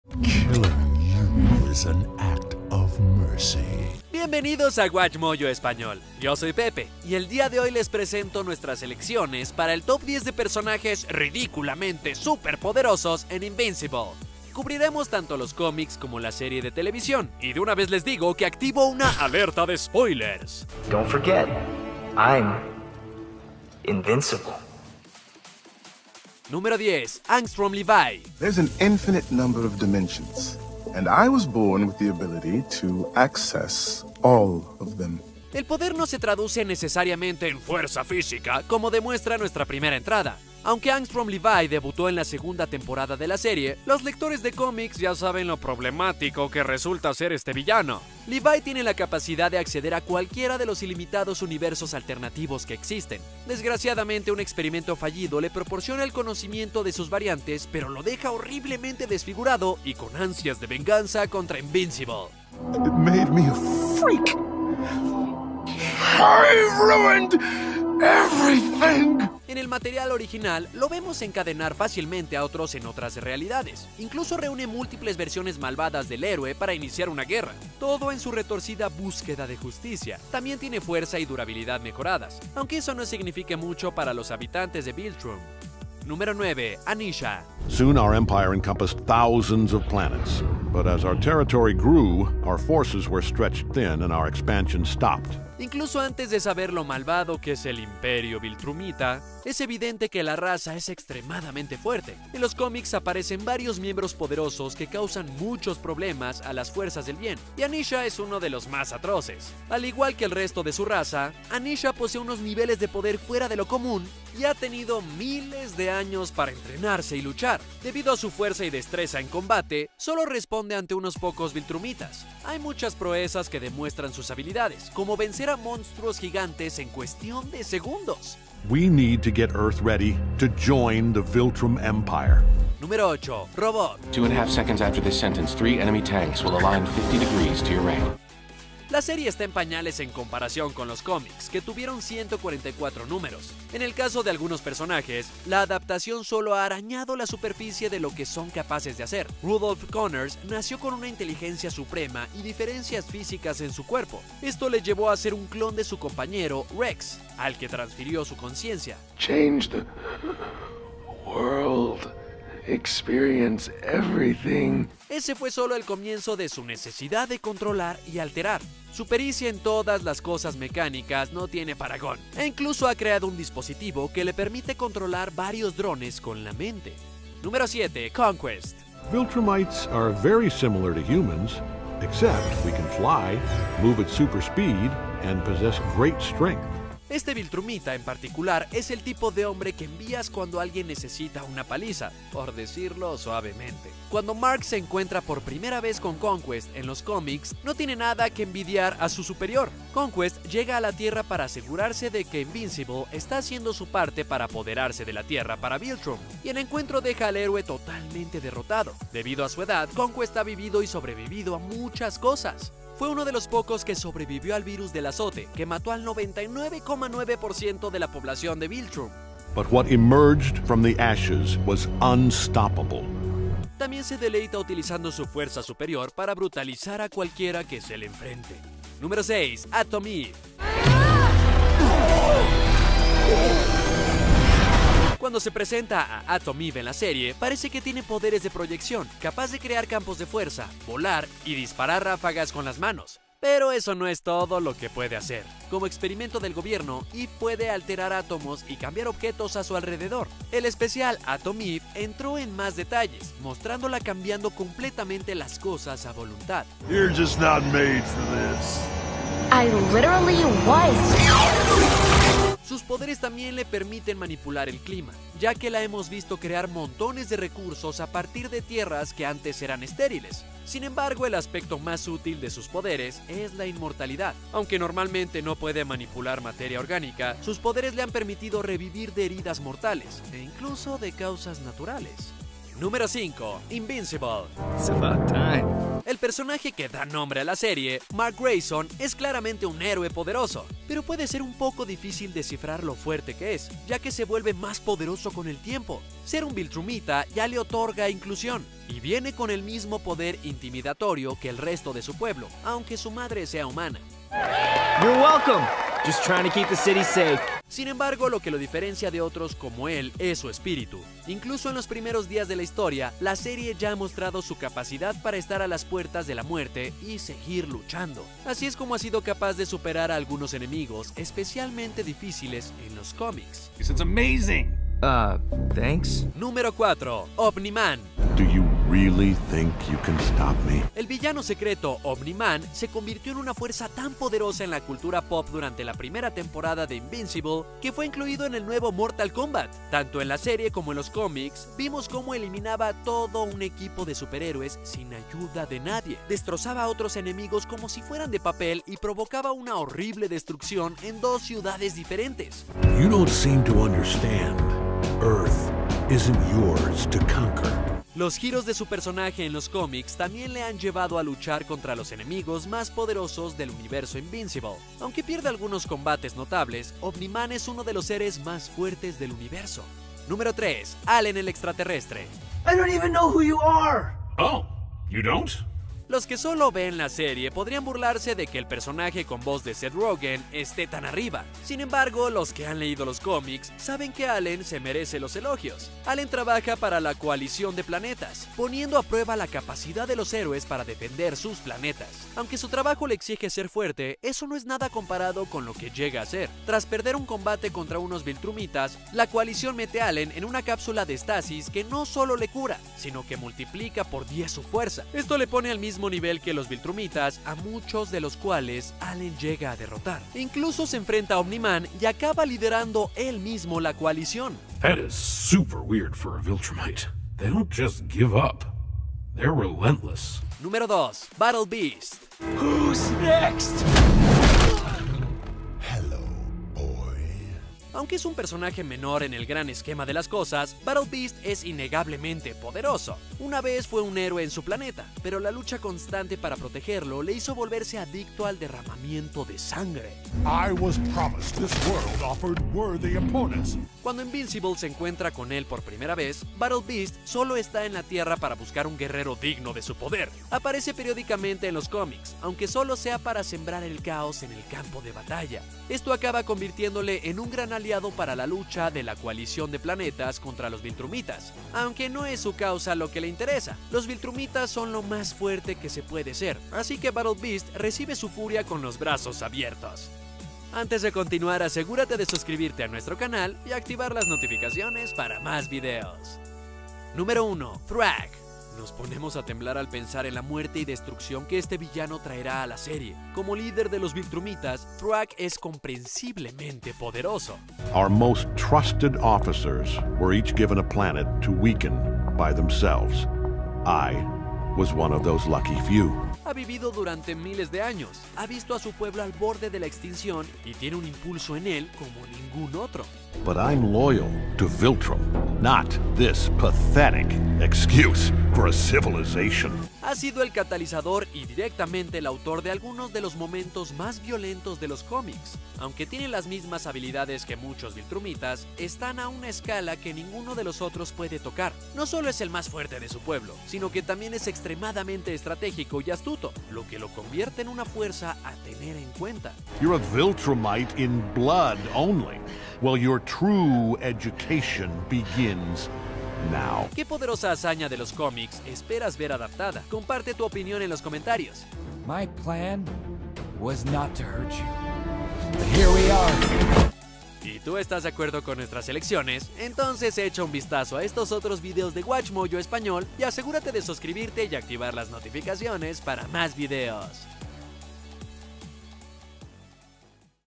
¡Top 10 Personajes RIDÍCULAMENTE Superpoderosos en Invincible!
Alerta de spoiler para los que no hayan leído los cómics. Bienvenidos a WatchMojo, y hoy hacemos un recuento de nuestras selecciones de los héroes y villanos más poderosos de "Invencible". Nuestra cuenta atrás de los personajes estúpidamente superpoderosos de "Invencible" incluye a Atom Eve, Omni-Man, Thragg, ¡y más!